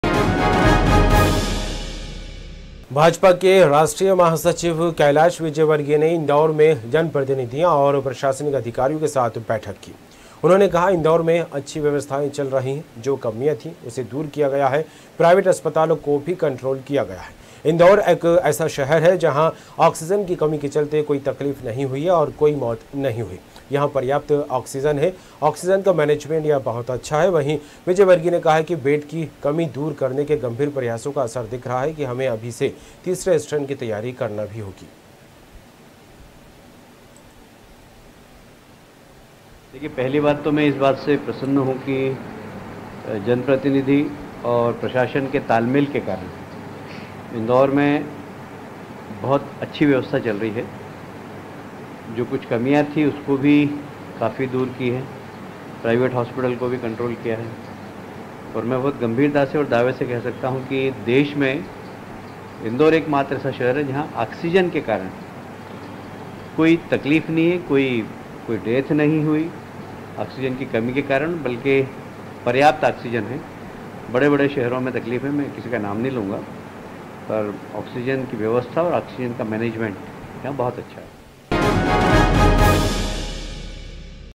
News World - इंदौर - भाजपा के राष्ट्रीय महासचिव की बैठक !
vijayvargeey।avb।इंदौर।मध्यप्रदेश।अनंत।7।5

भाजपा के राष्ट्रीय महासचिव कैलाश विजयवर्गीय ने इंदोर में जनप्रतिनिधियों ओर प्रशासनिक अधिकारियों के साथ बेठक की. जहां उन्होंने कहा... इंदोर में अच्छी व्यवस्थायें चल रही है जो कमियां थी उसे दूर किया गया है. प्राइवेट अस्पतालों को भी कंट्रोल किया गया है .इंदोर एक ऐसा शहर है जहां ऑक्सीजन की कमी के चलते कोई तकलीफ नही हुई है और कोई मौत नही हुई है यहां पर्याप्त ऑक्सीजन है. ऑक्सीजन का मैनेजमेंट यहां बहुत अच्छा है वहीं विजयवर्गीय ने कहा... बेड की कमी दूर करने के गंभीर प्रयासों का असर दिख रहा है हमें अभी से तीसरे स्ट्रेन की तैयारी करना होगी

बाईट - कैलाश विजयवर्गीय,राष्ट्रीय महासचिव, बीजेपी

भाजपा के राष्ट्रीय महासचिव की बैठक

 कैलाश विजयवर्गीय ने की बैठक

 जनप्रतिनिधियों और अधिकारियों के साथ बैठक की

 कहा-इंदौर में अच्छी व्यवस्थायें चल रही है

\\input3-pc\ftp\MAY 2021\07 MAY\VIDEO\kailash vijayvargiy